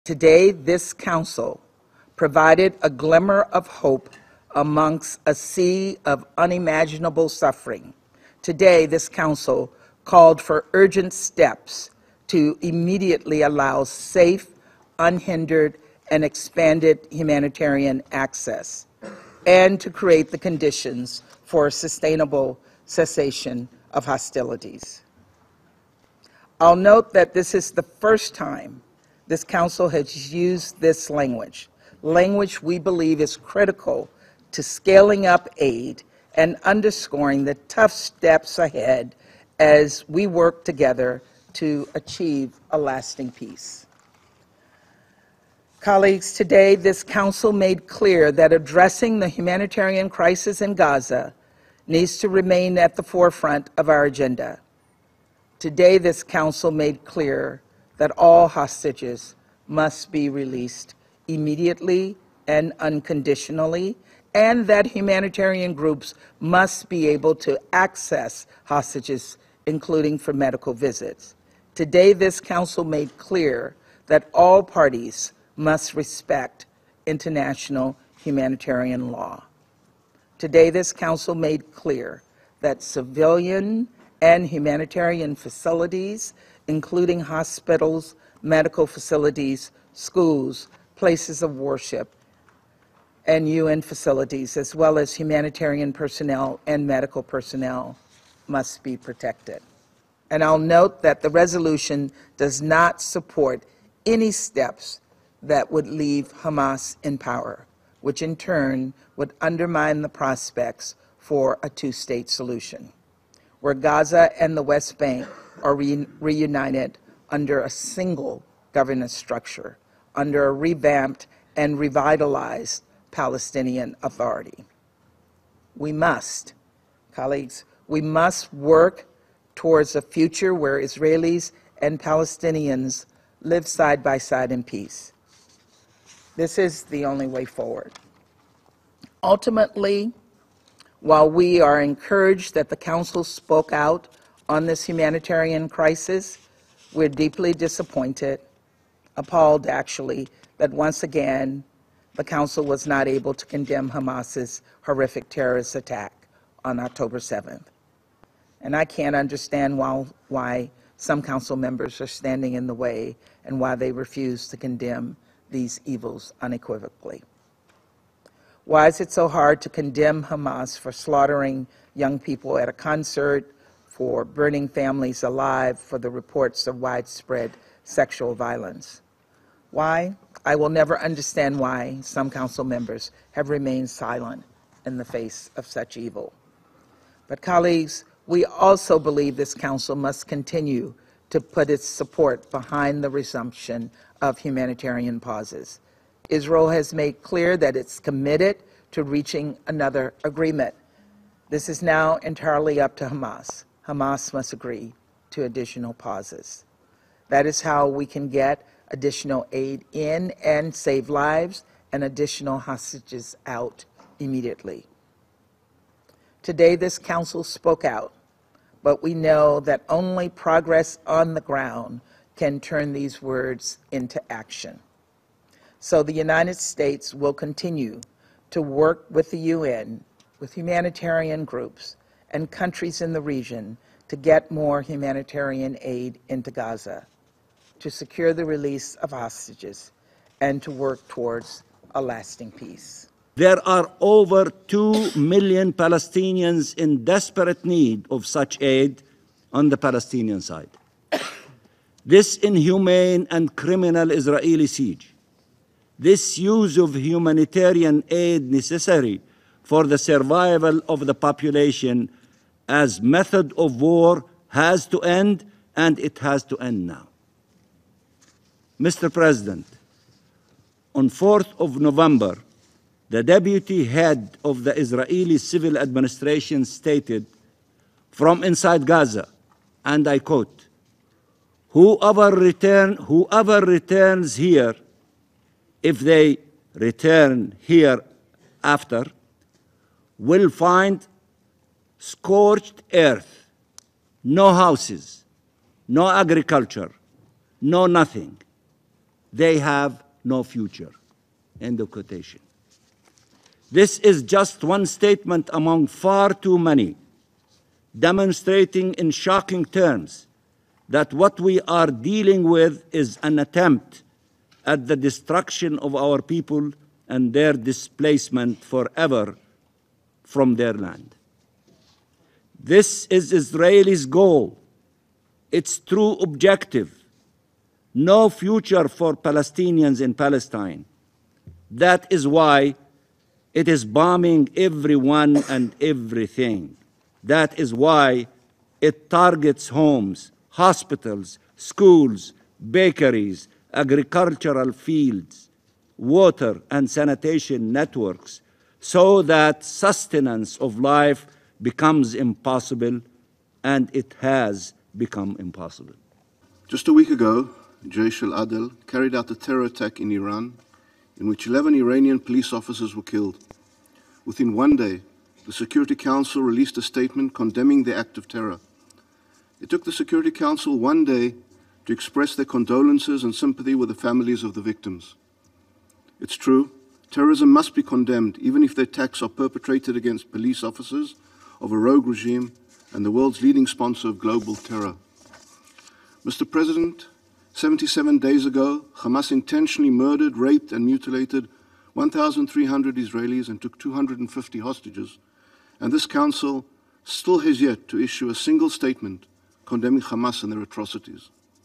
Israel-Gaza: UN Security Council passes resolution on expanding humanitarian aid
The UN Security Council adopted a watered-down resolution Friday calling for immediate speeded-up aid deliveries to hungry and desperate civilians in Gaza — but without the original call for an “urgent suspension of hostilities” between Israel and Hamas.

U.S. representative Linda Thomas-Greenfield said the resolution provides a "glimmer of hope."

"It is the first time council has used this language to scale up aid."

She said the vote bolsters efforts “to alleviate this humanitarian crisis to get life-saving assistance into Gaza and to get hostages out of Gaza, to push for the protection of innocent civilians and humanitarian workers and to work towards a lasting peace.”